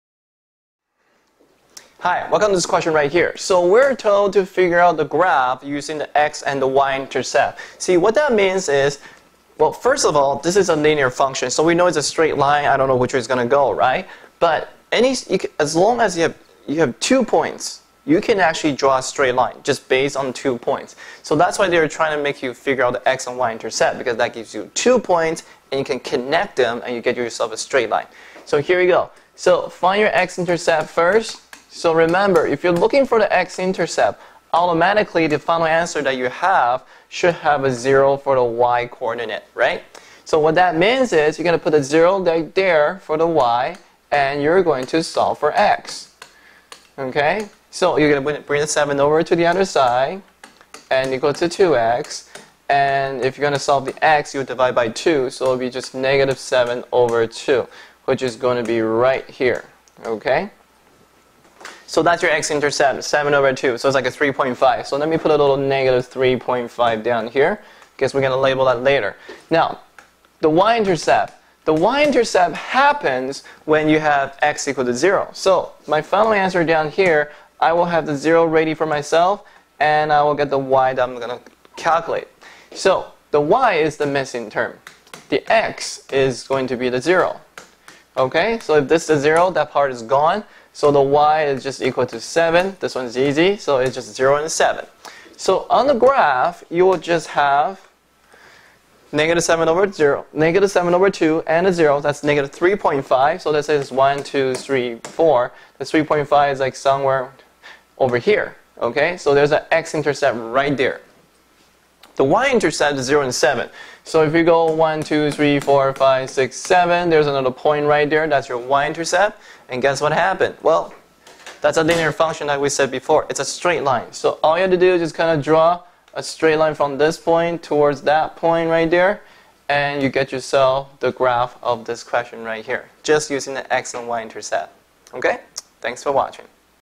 ʕ•ᴥ•ʔ Quickly Learn how to Graph a Line using the X Intercept & Y Intercept
Quickly master how to graph a function using the x- and y-intercepts.

One of the ways to graph a linear function is by using the x-and y-intercepts. If we know any two points of a straight line, it’s just a piece of cake to determine its equation and graph. In order to do that, we need to find out those intercepts by solving the function first.

Hi, welcome to this question right here.So we're told to figure out the graph using the x and y intercept. See, what that means is, well, first of all this is a linear function, so we know it's a straight line, I don't know which way it's gonna go, right. But, as long as you have two points, you can actually draw a straight line just based on two points. So that's why they're trying to make you figure out the x and y intercept, because that gives you two points and you can connect them and you can give yourself a straight line. So here we go; so find your x intercept first, so remember if you're looking for the x intercept, automatically the final answer that you have, should have a zero for the y coordinate, right. So what that means is, you're gonna put a zero right here for the "y" and you're going to solve for x. So you're gonna bring the 7 over to the other side, and equal to 2x; and if you're gonna solve for x you'll divide by 2 so it'll be -7/2, which is gonna be right here, okay. So that's your x intercept 7/2 sounds like a 3.5. So let me put a little -3.5 down here guess we're gonna label that later. Now, the y intercept; the y intercept happens when you have x=0. So my final answer down here, I will have the zero ready for myself and I'll get the "y" down... I'm gonna calculate. So the y is the missing term, the x is going to be the zero, okay. So if this is zero, that part is gone; so the y is just equal to 7. This one is easy, so it's just 0 and 7. So on the graph, you'll just have - 7/2 and the 0; that's -3.5 so that is 1,2,3,4 and 3.5 is somewhere over here, okay. So there's the x intercept right there. The y intercept is 0 and 7. so if you go 1,2,3,4,5,6,7, there's another point right there, that's your y intercept and guess what happens. Well that's the linear function I always said before, it's a straight line. So all you have to do is just kinda draw a straight line from this point towards that point right there and you get yourself the graph of this question right here, just using the x and y intercept, okay.